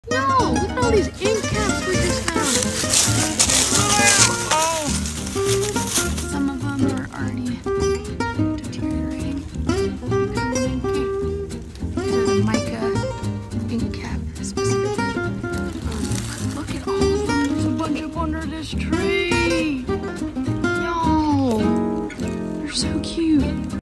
Spotted 👀 Ink Caps! 🖋️ 🍄
Mica Cap mushrooms, Coprinellus micaceus, grow in woodland areas as well as in urban settings, feeding off dead wood of broadleaf trees.